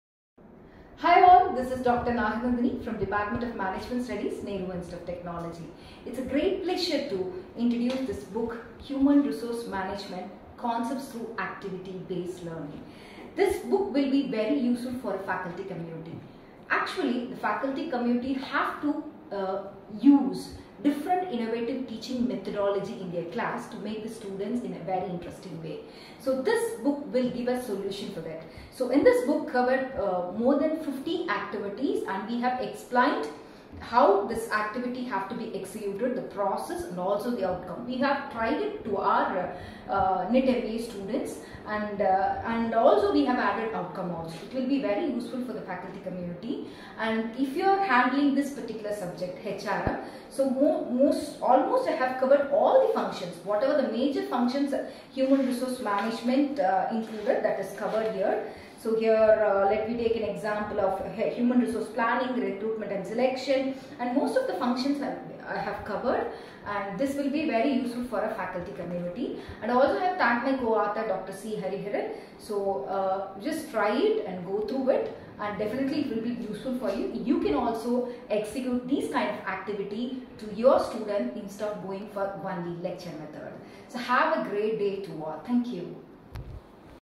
Human Resource Management book entitled: HRM Concepts Through Activity Based Learning
Active learning transforms the classroom into a space of exploration. Students actively participate, constructing knowledge through hands-on experiences, discussions, and collaboration”.
50 + Activities in HRM Book entitled “HRM Concepts Through Activity Based learning”
It’s time to use innovative teaching methods in your classroom….